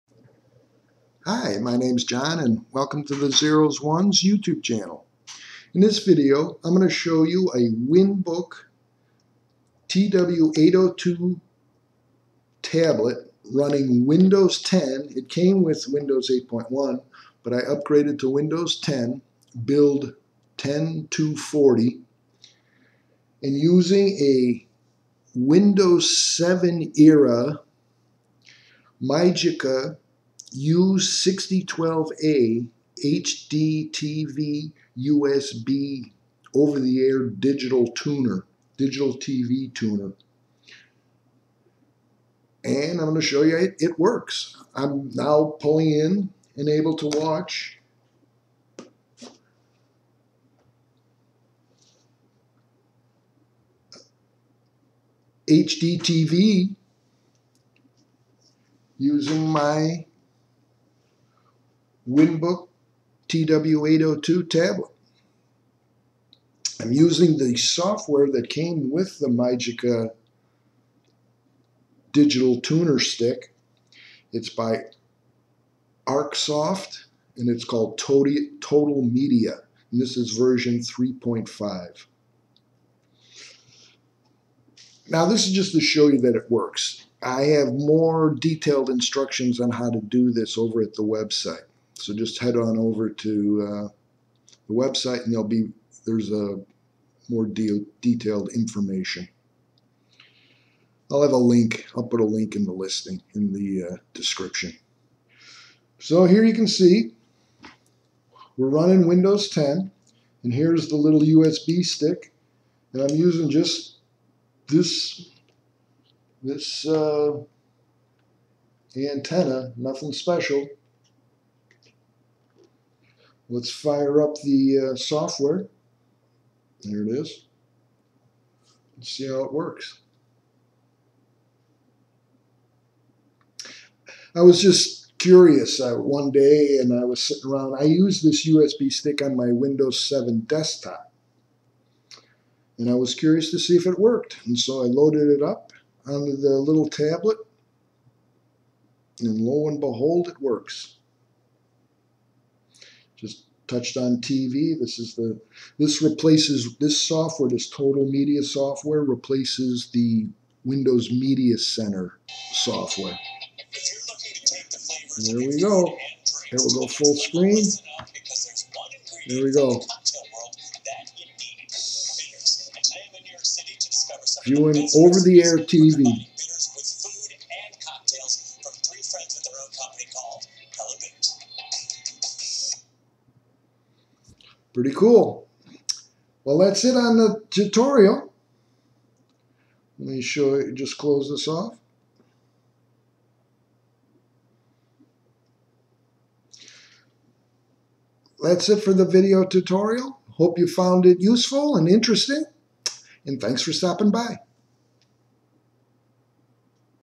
WinBook TW802 USB HDTV Tuner Watch OTA TV
WinBook TW802 USB HDTV Tuner Watch OTA TV shows how to setup receiving over the air HDTV on WinBook TW802 and Mygica U6012A